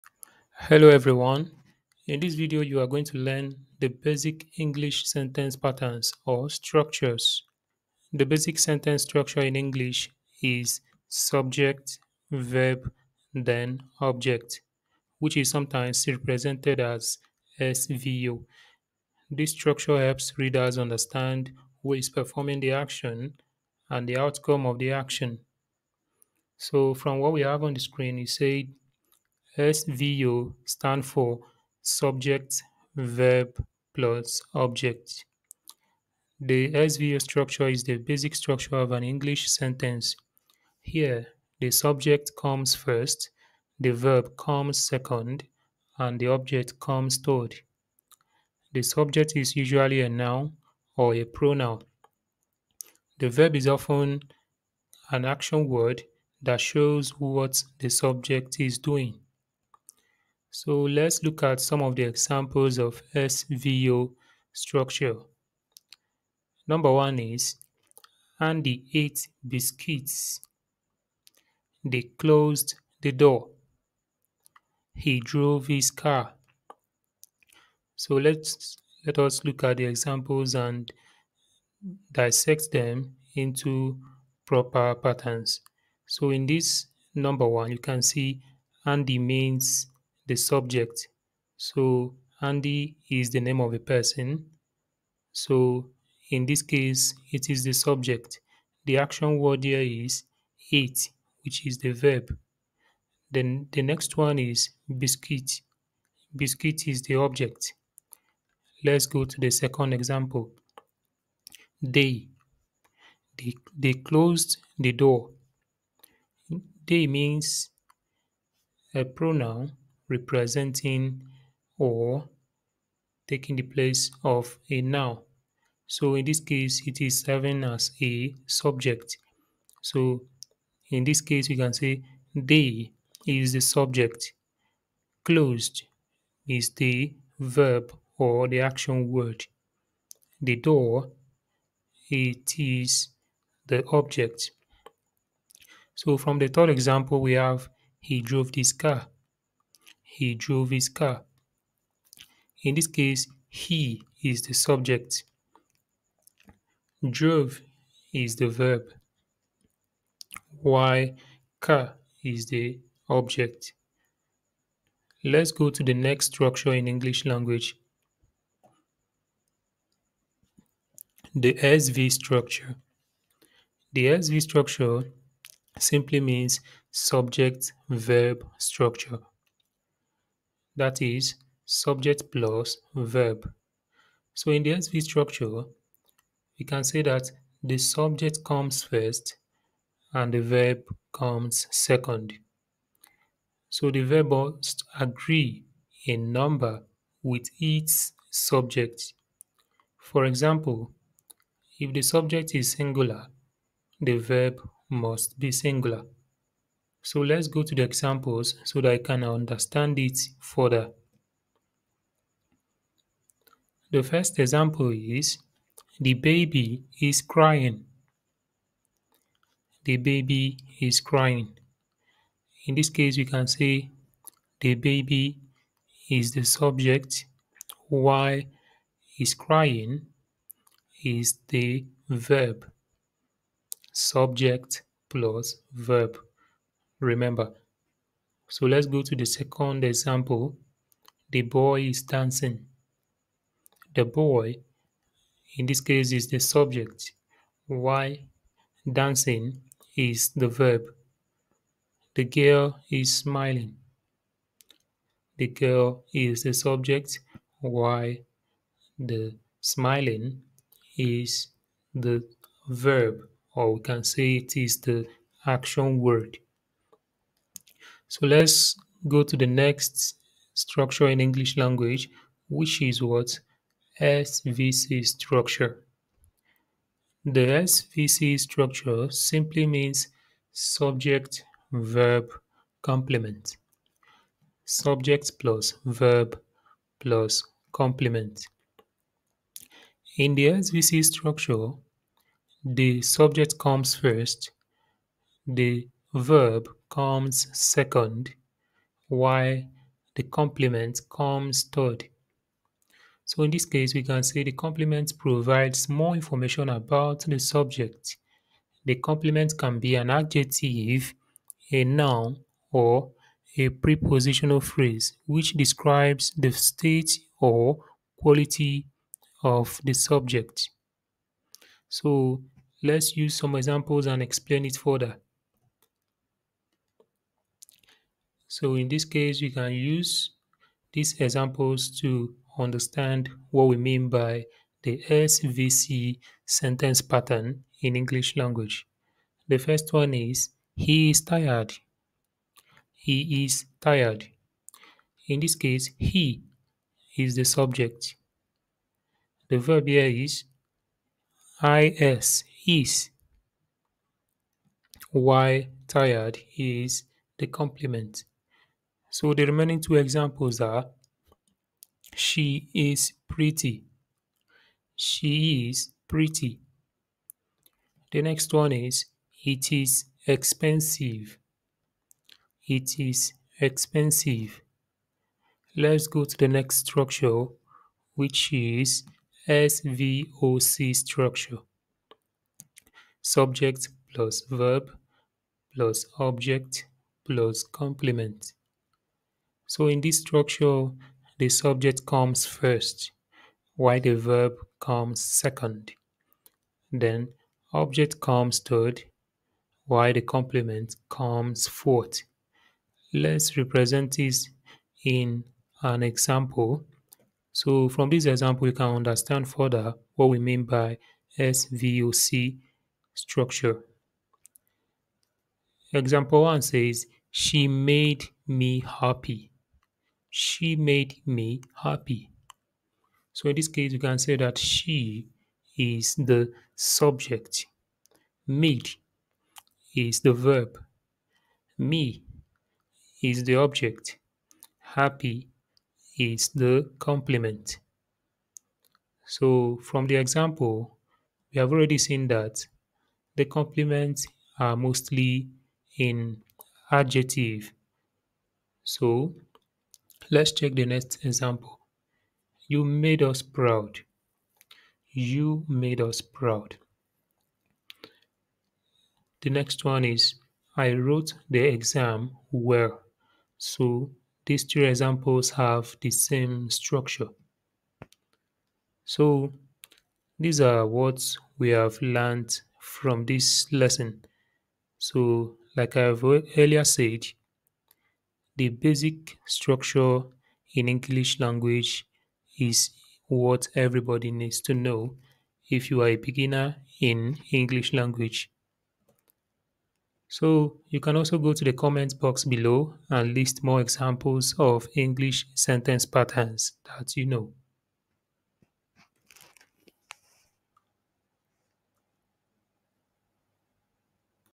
Master English Sentence Structure: Essential Patterns for Fluent Communication
Unlock the secrets of English sentence structure with our comprehensive guide! Whether you're a beginner or looking to refine your skills, this video covers everything you need to know about constructing clear and effective sentences in English. Learn essential patterns, common mistakes to avoid, and practical tips for confident communication. Start building strong foundations for fluent English today!